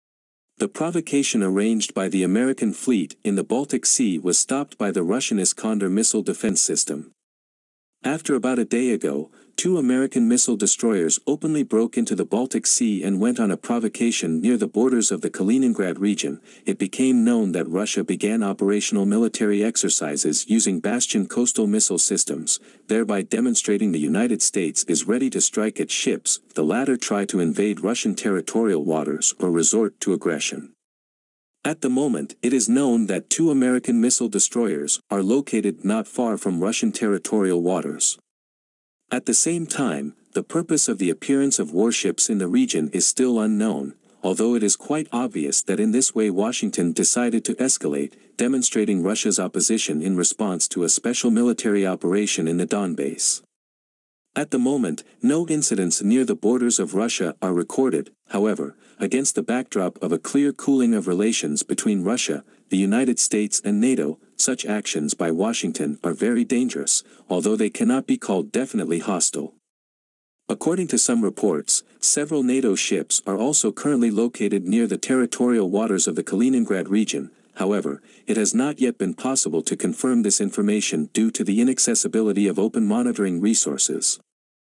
Russia met American missile destroyers approaching Kaliningrad with Bastion complexes
The provocation arranged by the American fleet in the Baltic Sea was stopped by the Russian Iskander missile defense system.

After about a day ago, two American missile destroyers openly broke into the Baltic Sea and went on a provocation near the borders of the Kaliningrad region, it became known that Russia began operational military exercises using Bastion coastal missile systems, thereby demonstrating The United States is ready to strike at ships if the latter try to invade Russian territorial waters or resort to aggression.